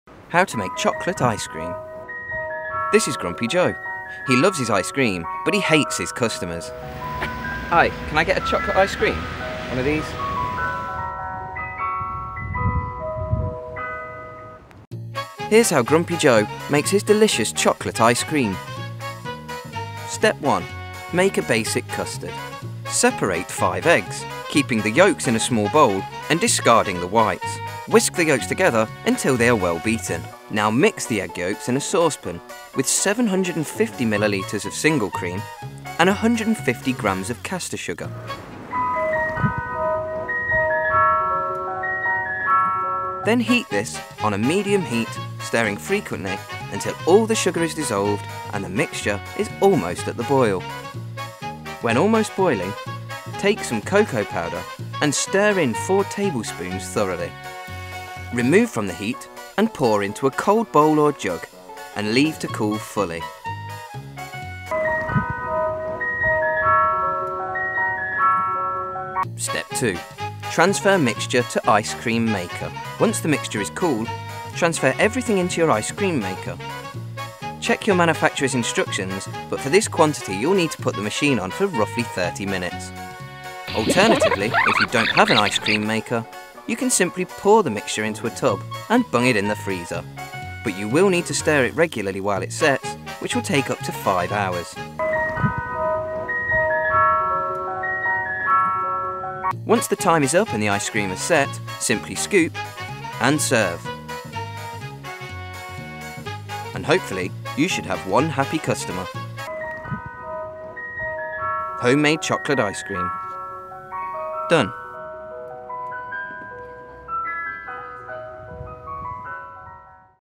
How To Prepare Homemade Chocolate Ice Cream
Watch How To Prepare Homemade Chocolate Ice Cream from the how to specialists. This tutorial will give you useful instructions to ensure you get good at chocolate recipes, ice cream, cooking for kids.